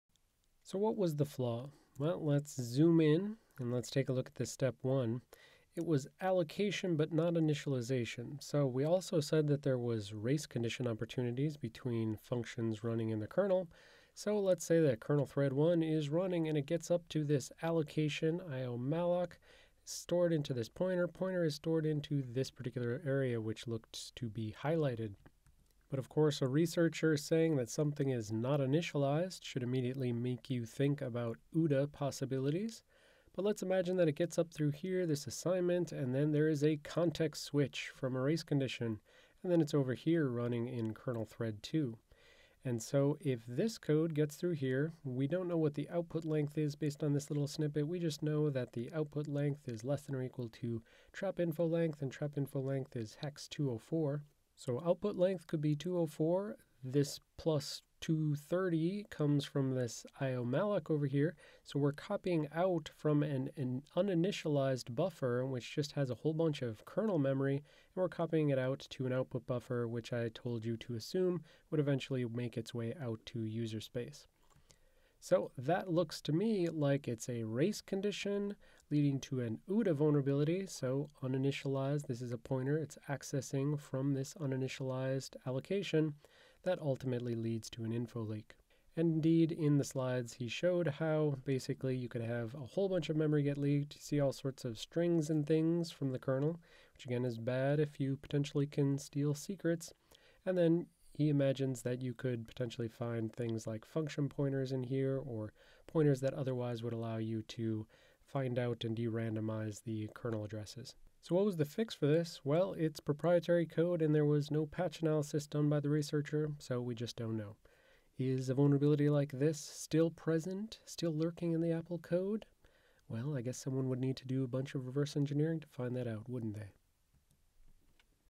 Vulnerabilities 1002: 06 Information Disclosure CVE-2020-9833🍎📶 02 FTF Answer
This class is for C/C++ developers learning secure development, and vulnerability hunters learning how to detect Uninitialized Data Access (UDA), Race Conditions (e.g. double fetch, Time of Check, Time of Use (TOCTOU)), Use After Free (UAF), Type Confusion, and Information Disclosure.